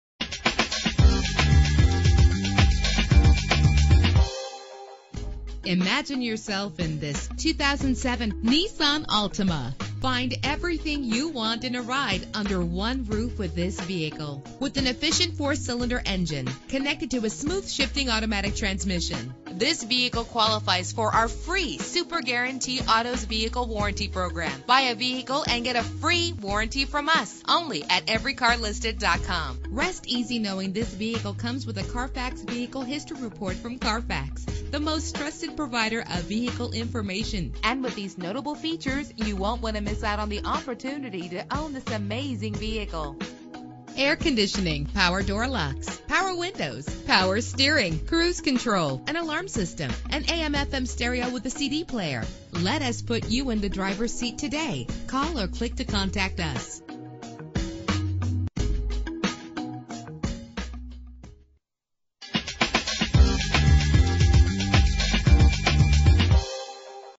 2007 Nissan Altima - Industry CA
Year: 2007
Make: Nissan
Model: Altima
Trim: 2.5 S
Engine: 2.5 liter inline 4 cylinder 16 valve
Transmission: Automatic CVT
Color: Black
Mileage: 53815
Address: